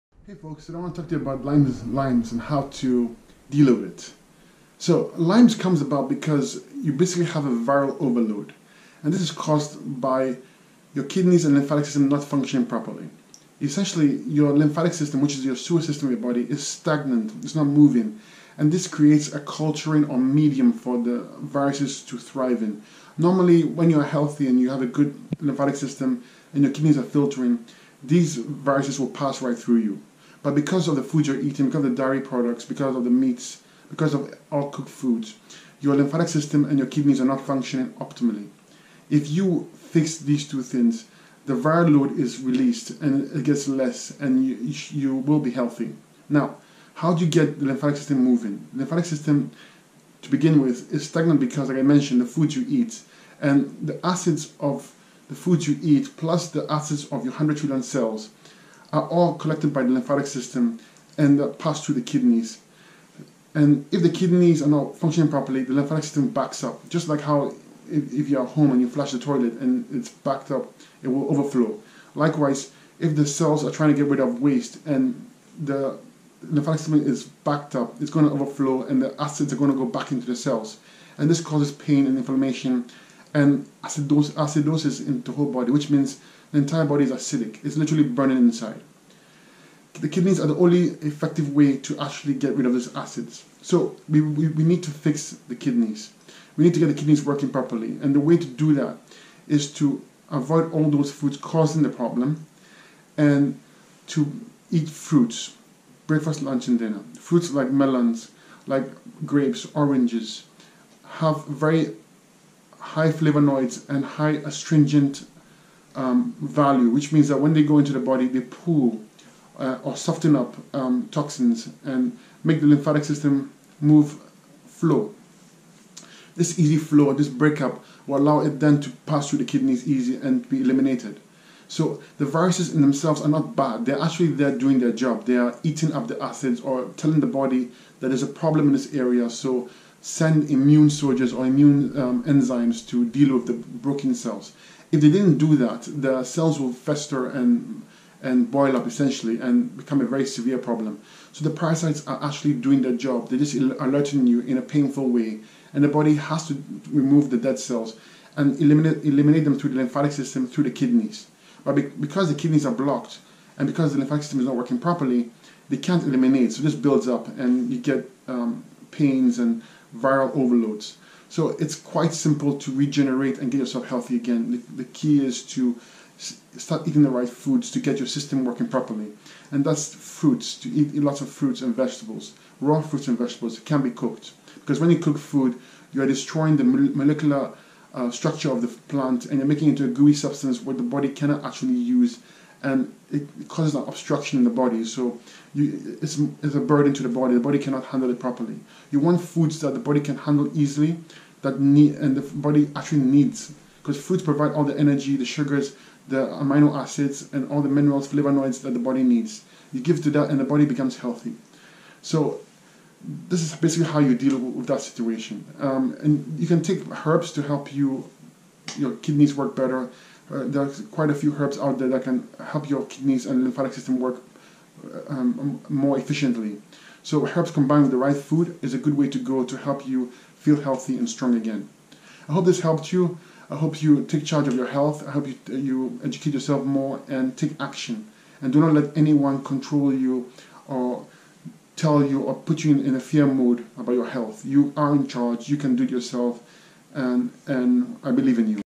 Detoxing lymes and getting healthy
Here is how to thrive with lymes. Know how to deal with it and live a healthy vibrant life